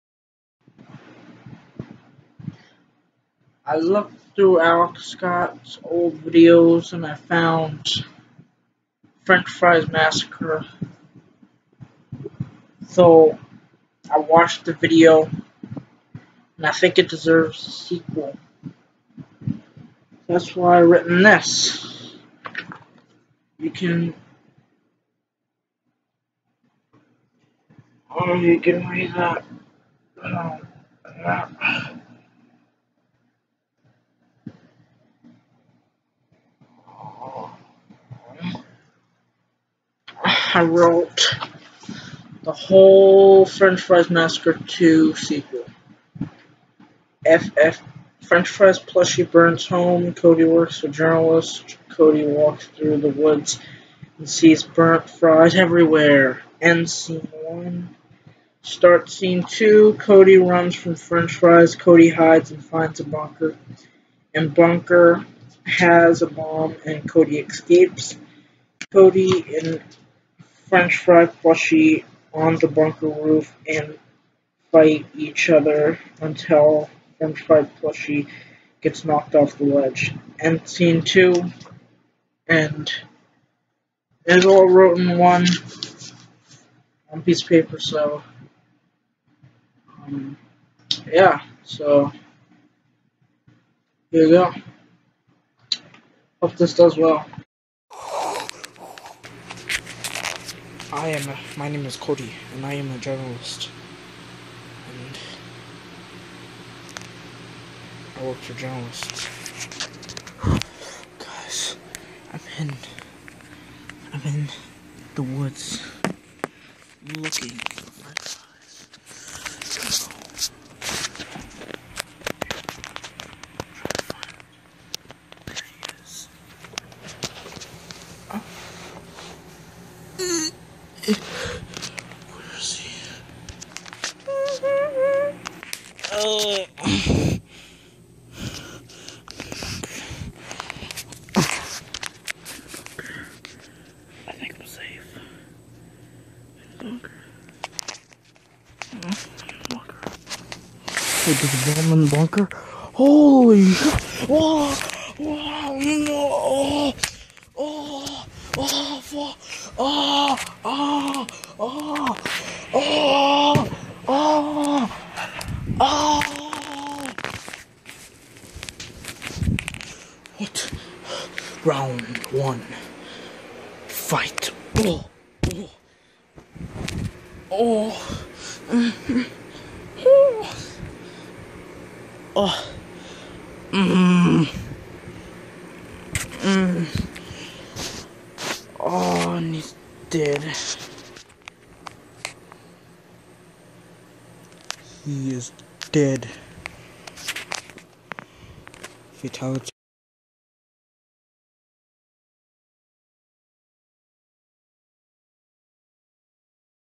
is this the end of the road?
Drew Howard loses ideas until watching French Fries Massacre, then decides to do a sequel.

Alex Scott 2017
Drew Howard Series

My channel content revolves around vlogs, skits, short films, parodies, and more!